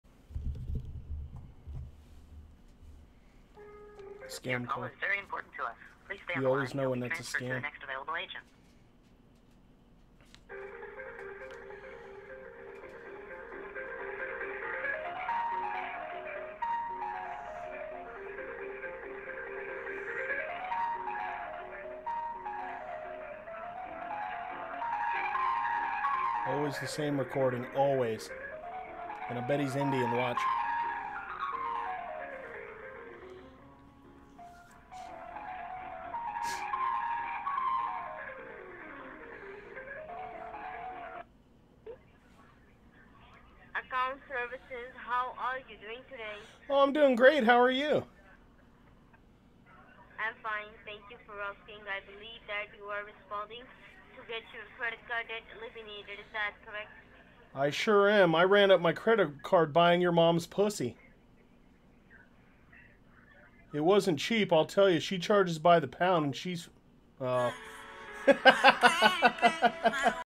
Messing With Another Indian Scammer Phone Call
this bitch, shes no fun